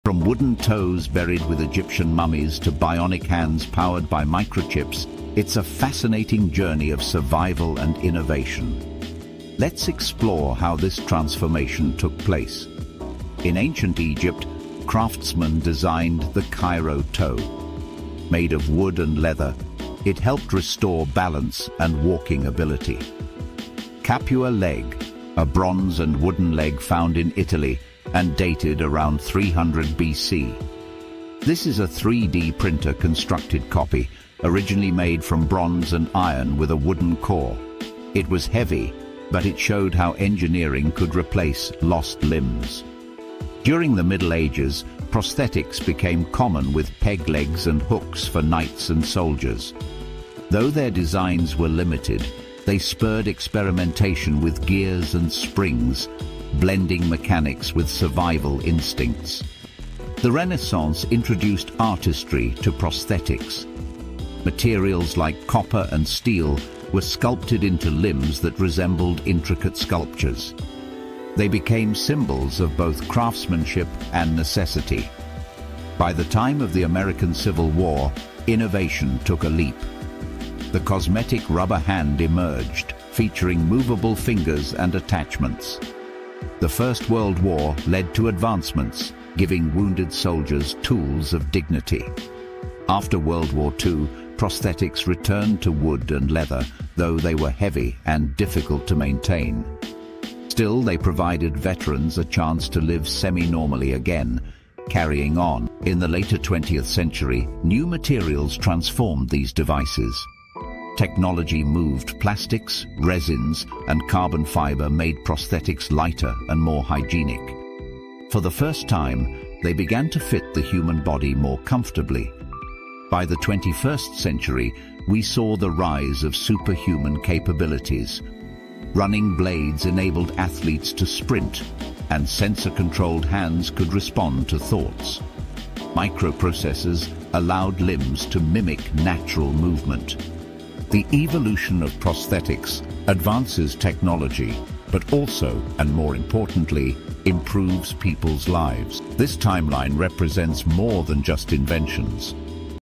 3000 Years of Prosthetics: From Mummies to Bionics
Discover the incredible journey of prosthetics — from ancient wooden toes buried with Egyptian mummies to advanced bionic hands powered by microchips. Across centuries, prosthetics evolved from survival tools into superhuman technology. Cairo Toe, Capua Leg, medieval hooks, Civil War rubber hands, WWII leather limbs, carbon fiber blades, and sensor‑controlled hands — this timeline shows resilience, identity, and innovation.